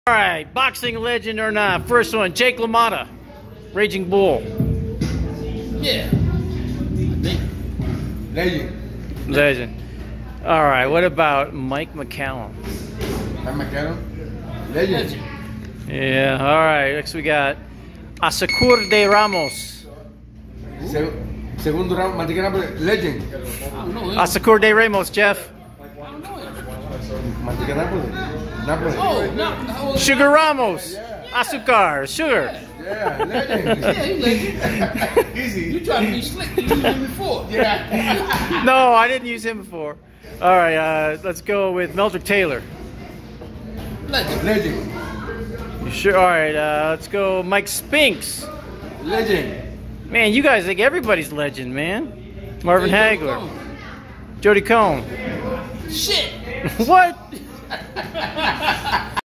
Boxing Legend or No Legend? Part 3 w/ Jeff Mayweather & Joel Casamayor
Jeff Mayweather and Joel Casamayor say whether or not they think these boxers qualify as "legends."